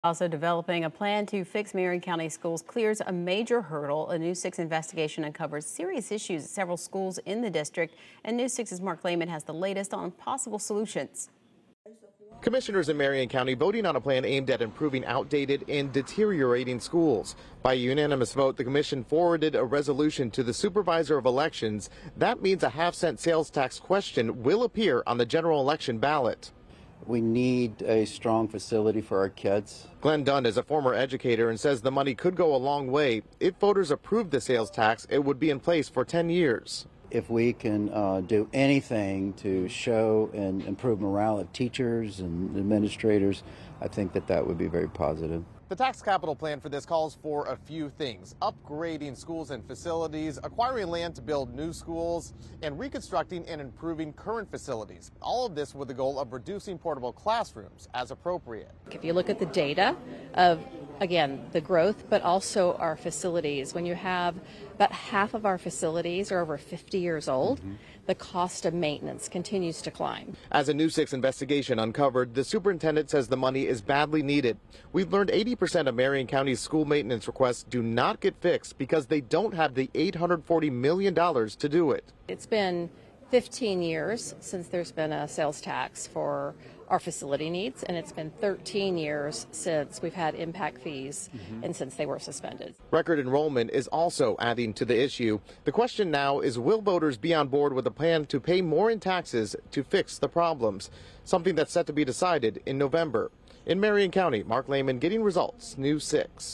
Voters to decide on half-cent sales tax for Marion County Schools
Marion County commissioners have pushed forward a resolution from the school board for a half-cent sales tax to appear on the general election ballot.